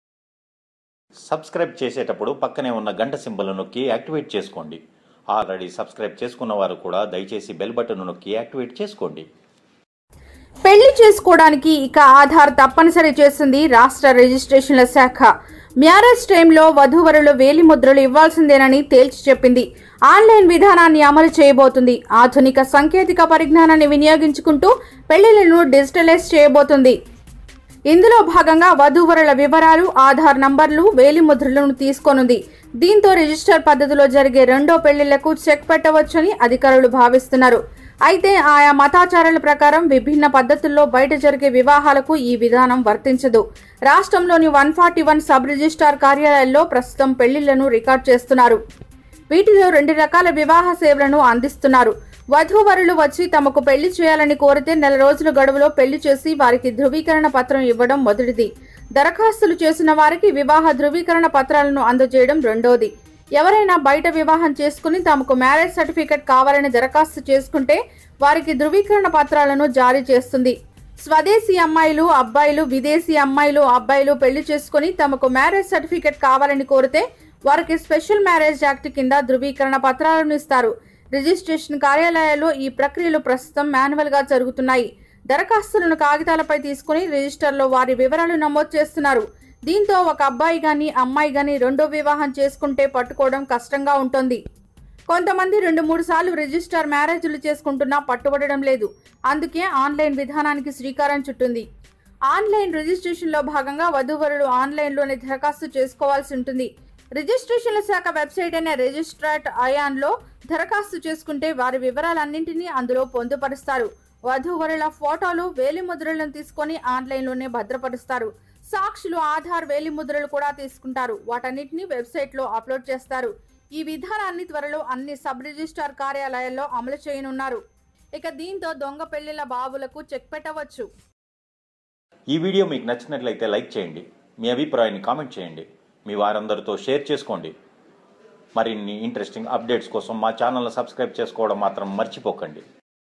Aadhaar Is Mandatory For Marriage - Newsmarg.com
The Delhi government has already decided to make aadhaar mandatory for people if they want to register their marriage and property. The state of Andhra Pradesh is also walking on the footsteps of Delhi and have made aadhaar mandatory for marriage. The registration department has also been informed about the new changes that are brought to the system.